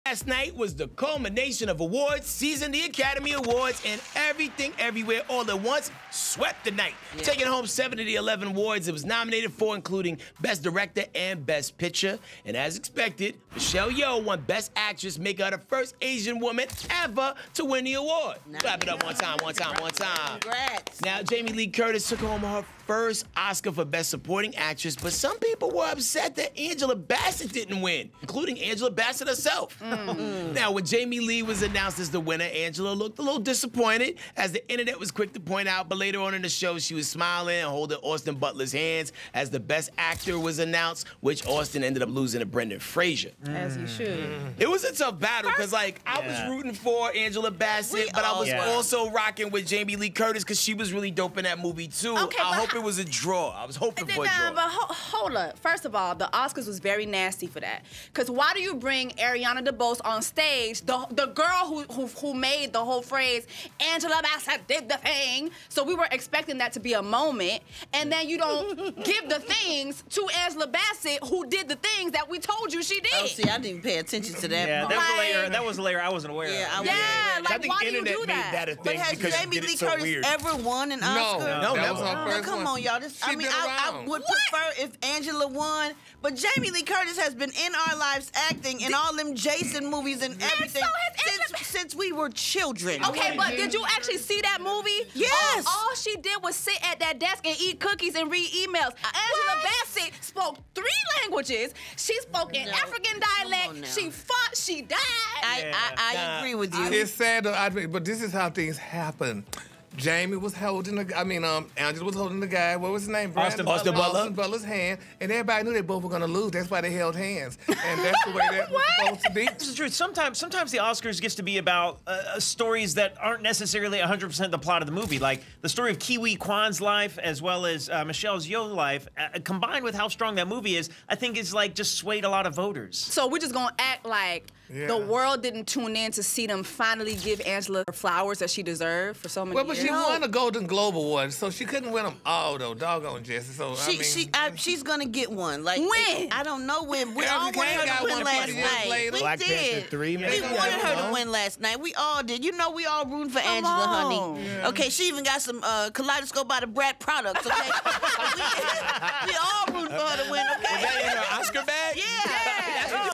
Angela Bassett Gets Snubbed Again at Oscars—Jamie Lee Curtis Snags Best Supporting Actress
Angela Bassett gets snubbed again at The Oscars!

TheOscars was in full effect last night with historical wins and some devastating blows! 'Everything Everywhere All at Once' swept, winning 7 of 11 noms with lead actress Michelle Yeo winning Best Actress making her the first Asian woman ever to win. Jamie Lee Curtis, who was up against AngelaBassett for BestSupportingActress, won her first-ever Oscar, beating Angela in the category and fans were outraged that Angela got snubbed by the Oscars for a second time! 🤬⁠
DishFam, what did you think about Angela getting snubbed for the second time by the Oscars? ⁠